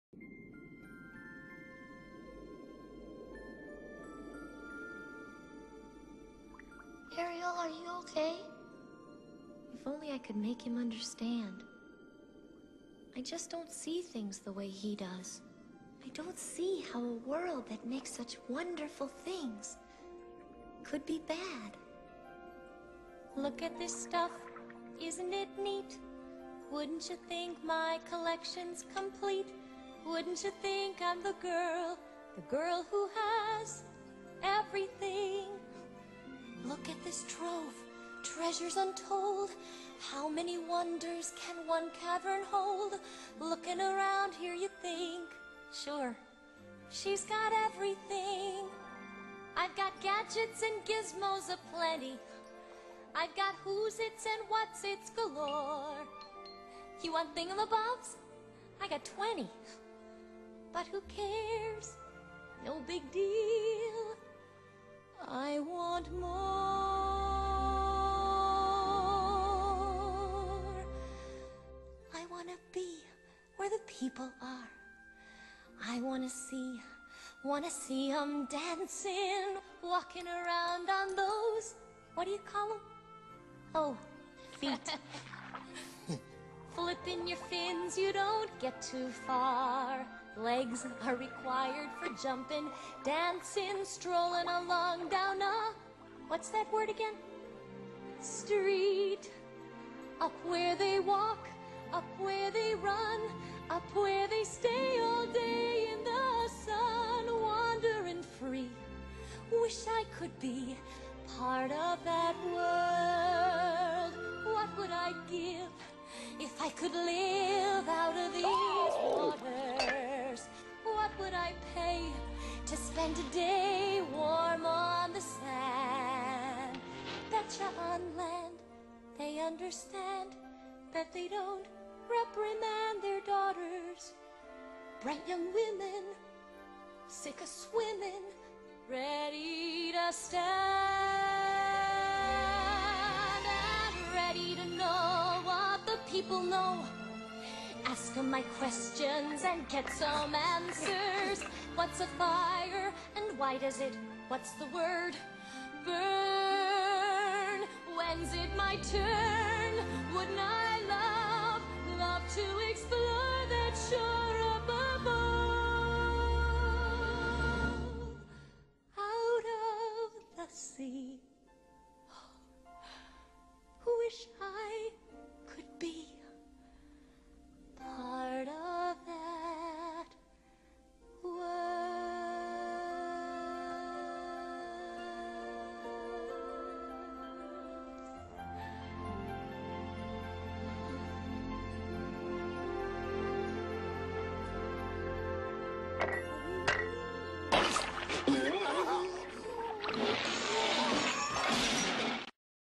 Little Mermaid Speed Painting
this is a painting of the little mermaid. colours and background are all own choice.